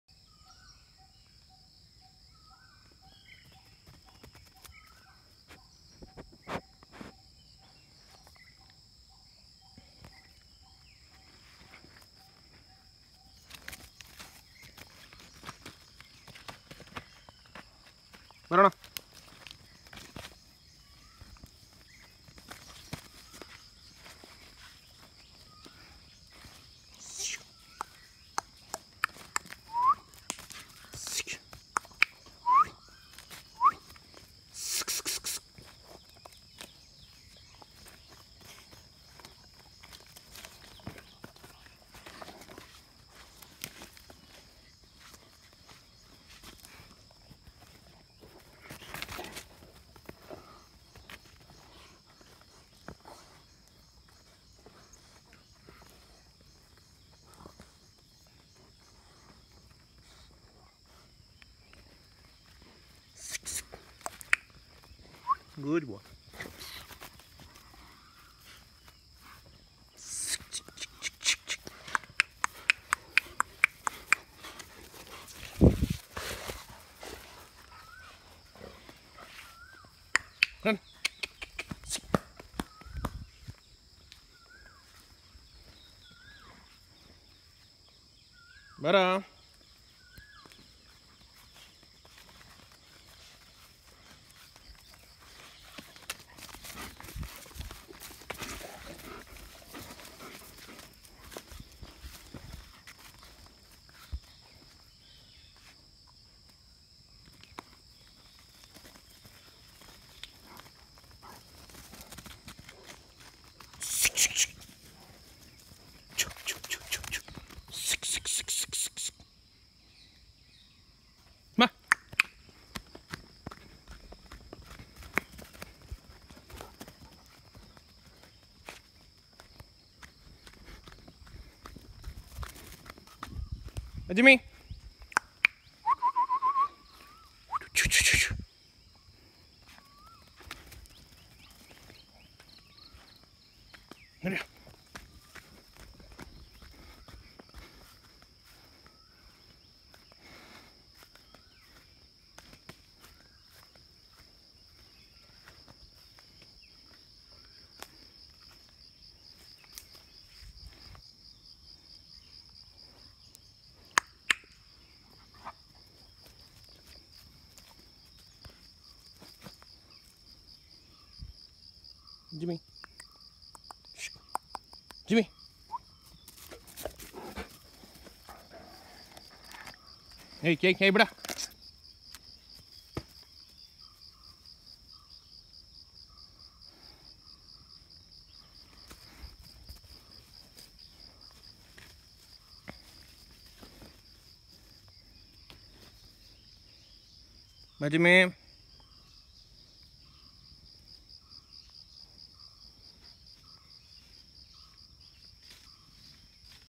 Fog at our Estate. Early Morning Routine | Great Dane & Mudhol Hound dogs playtime!
Morning Routine of my Dogs' Great Dane RANA and Mudhol Hound JIMMY at our Estate.
Watch Hyperactive and playful dogs in Loving Nature.